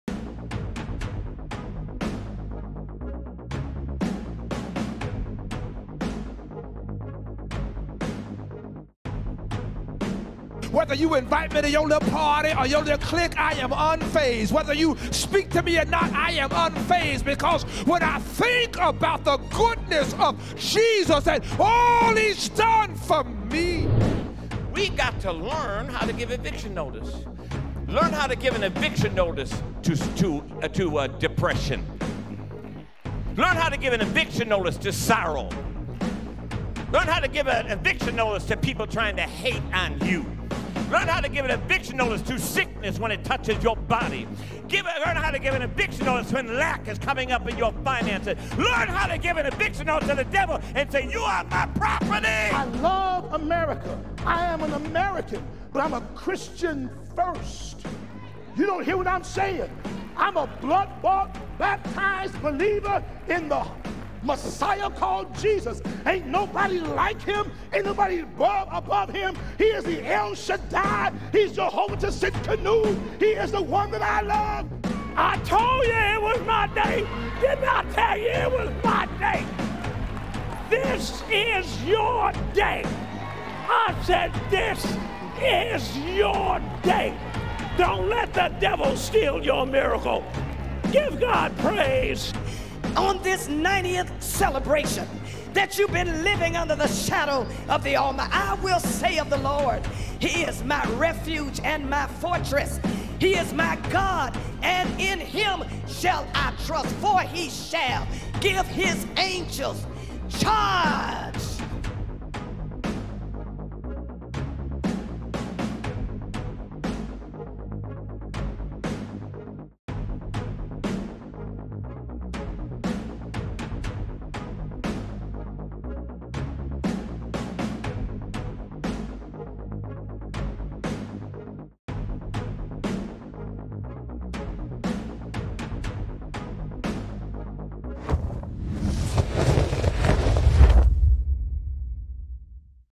90th Annual Bible Conference: Legacy of Truth Trailer
Praise the Lord! For 90 years The Apostolic Church of God has been sharing the gospel and lifting up the name of Jesus and God’s grace has been upon Apostolic for 5 generations. Our theme “Legacy of Truth, Generations of Trusting God” was the focus of the powerhouse speakers who ministered at our 90th Annual Bible Conference. Watch the entire conference, or favorite individual speaker or Pre-Show interview with a one-time purchase of only $24.99 for unlimited access to stream or download video.